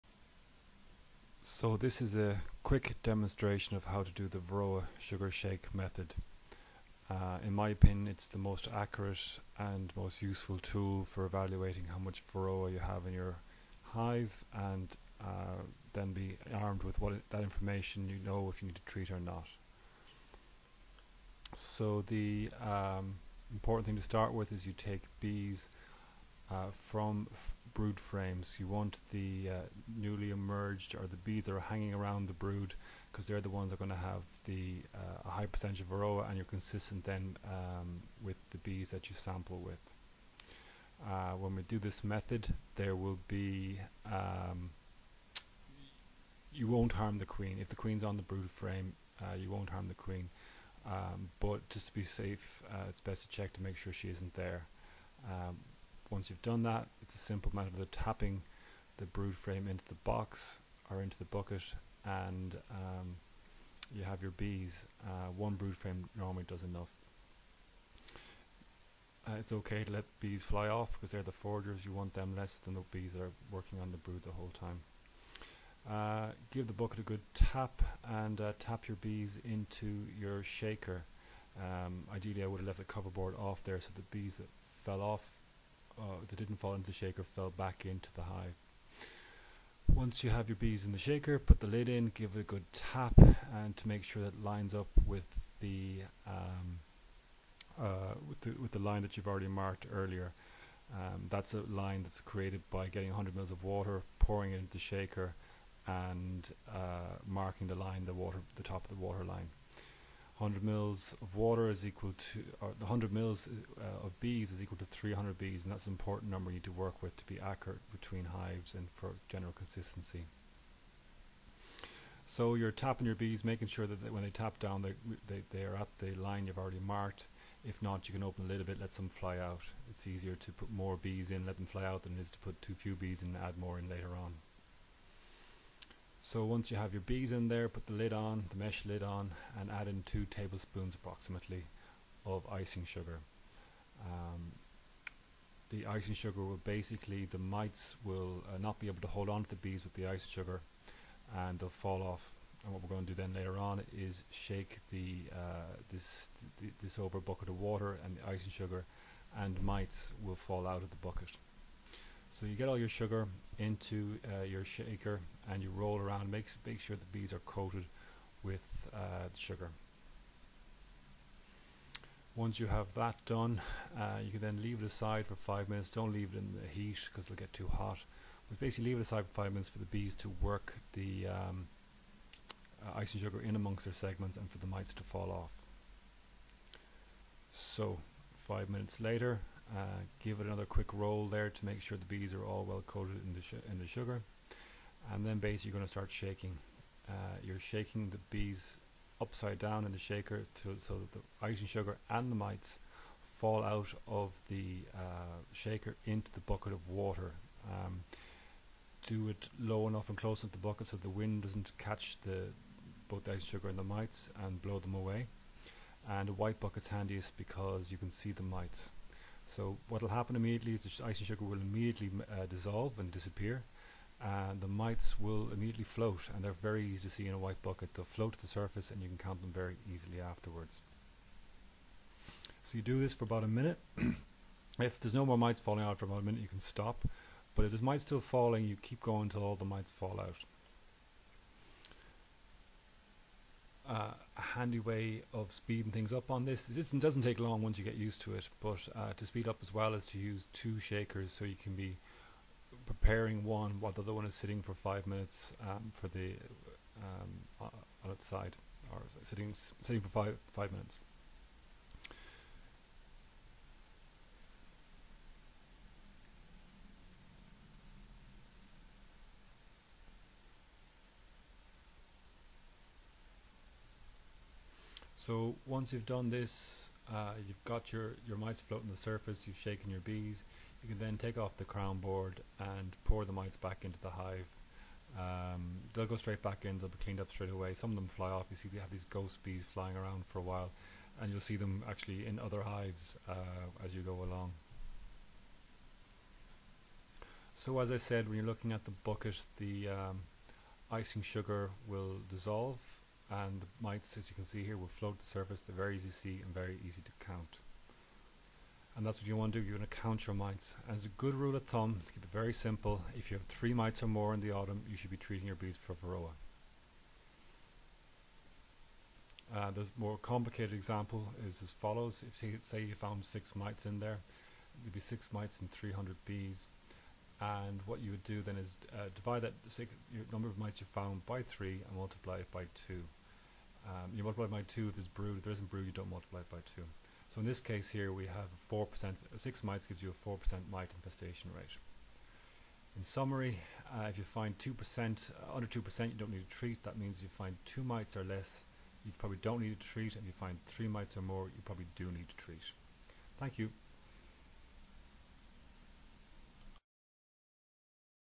Varroa sugar shake method
This is a short description of how to test accurately for Varroa. It is highly recommended to ensure you keep Varroa levels low in the hive which is probably the most important thing for colony health. You can also use this method to help decide which colonies to breed from by determining which are best at keeping Varroa levels low. Doing this can even save you money as if you find levels are low treatment may not be needed. I am also posting another video on the benefits of performing this method in particular focusing on Irish beekeeping.